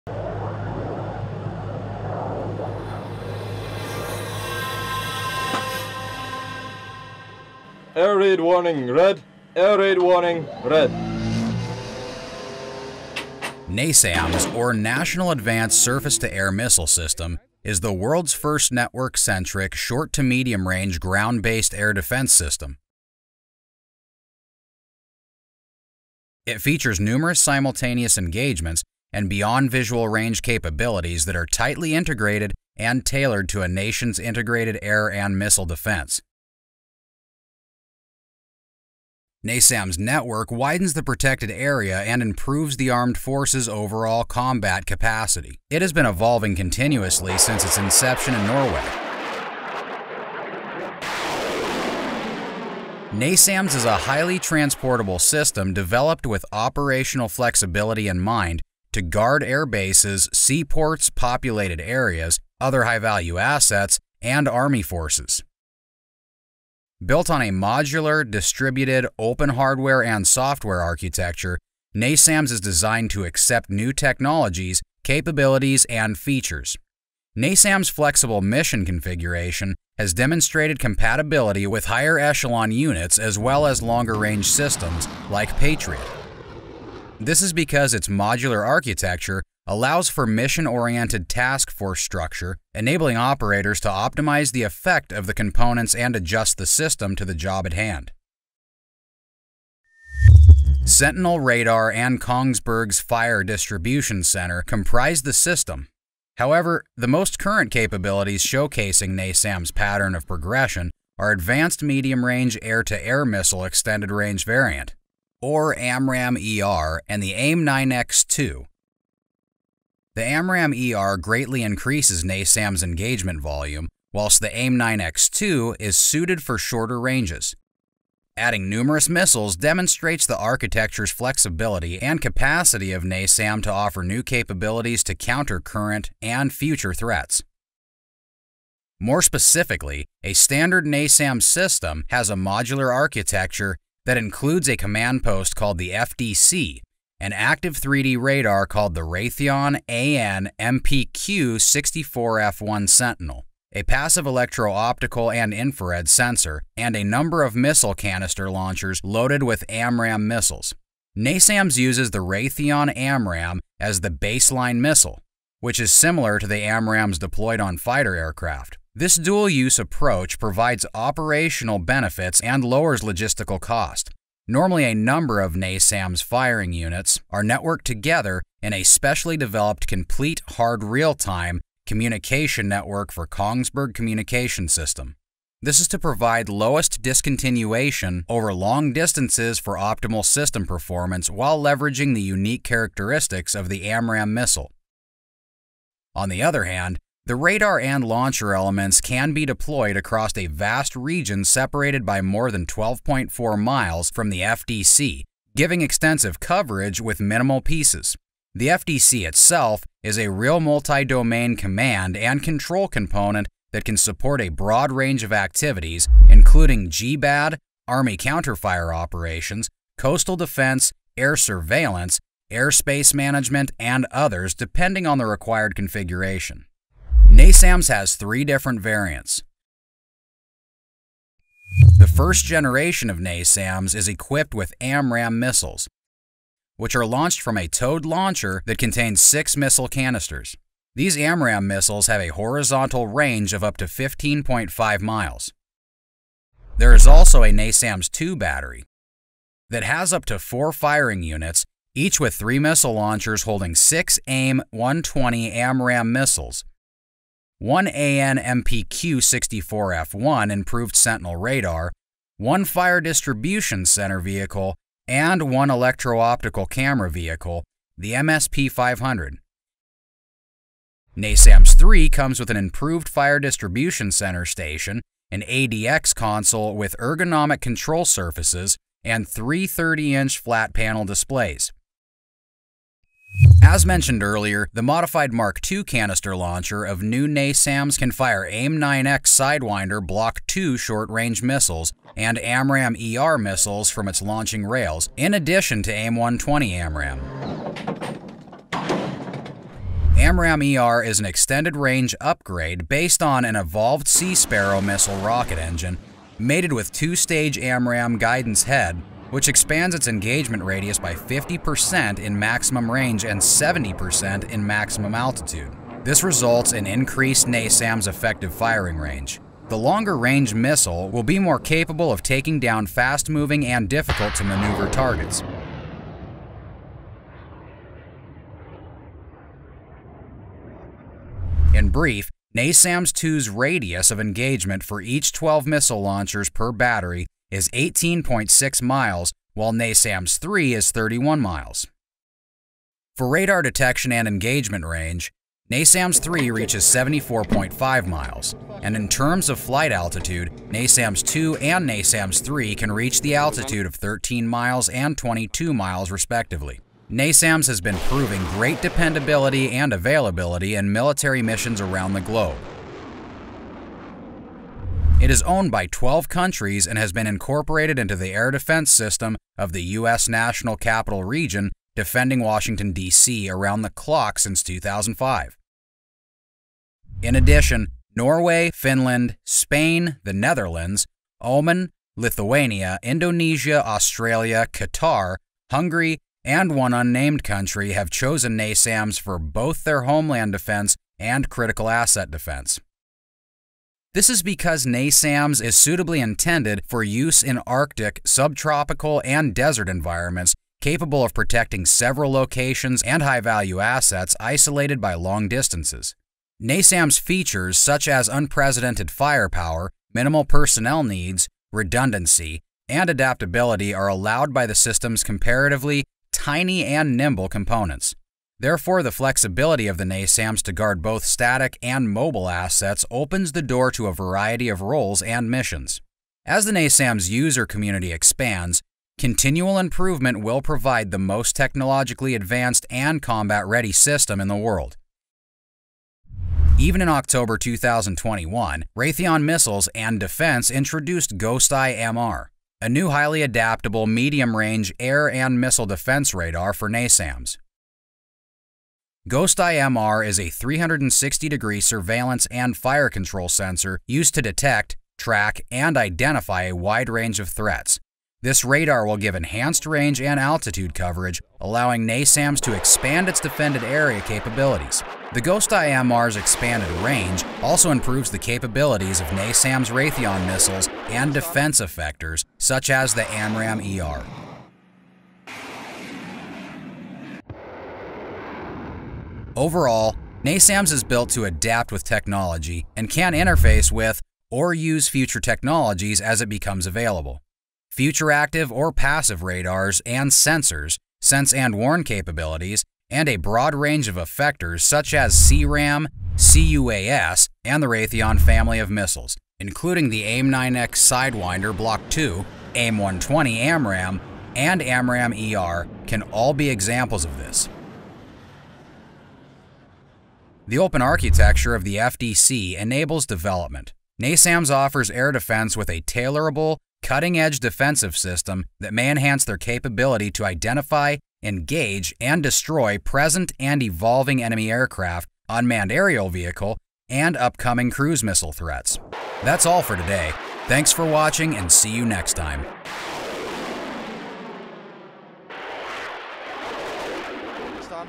How powerful is NASAMS Air Defence System
NASAMS or National Advanced Surface-to-Air Missile System is the world’s first network-centric short- to medium-range ground-based air defense system. It features numerous simultaneous engagements and Beyond Visual Range capabilities that are tightly integrated and tailored to a nation’s Integrated Air and Missile Defense. NASAMS network widens the protected area and improves the armed forces’ overall combat capacity. It has been evolving continuously since its inception in Norway.